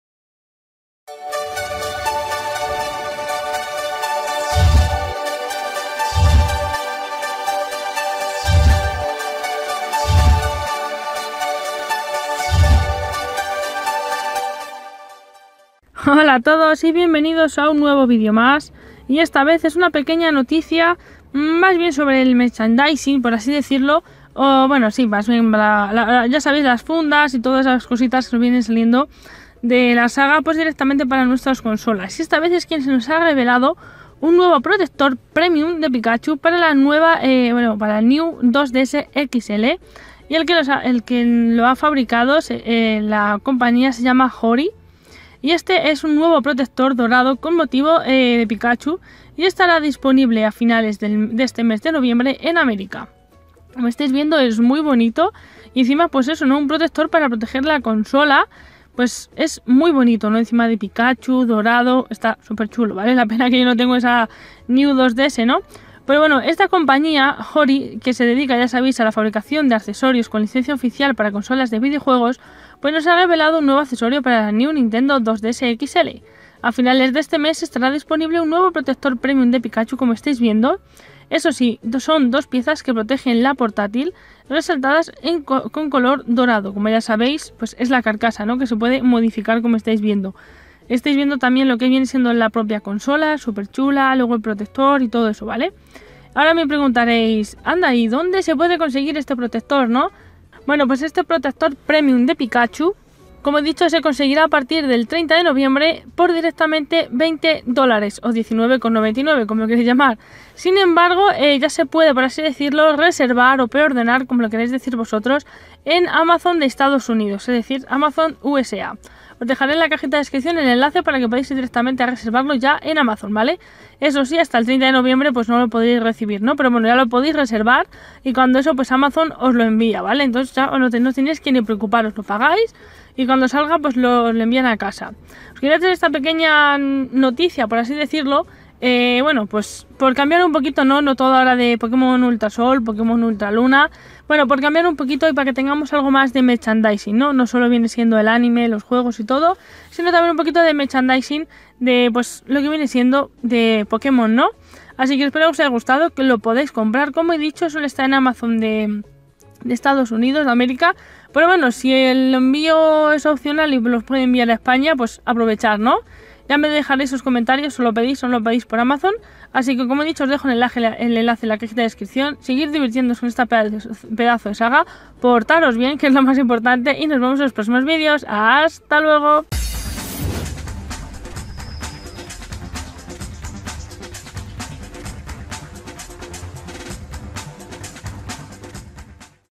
Revelado nuevo Protector Premium de Pikachu para New 2DS XL
Os cuento un poquito sobre el protector premium para la New 2DS XL, con motivo de pikachu.
Para reservarlo
Link de la aplicacion web donde se iran publicando las nuevas noticias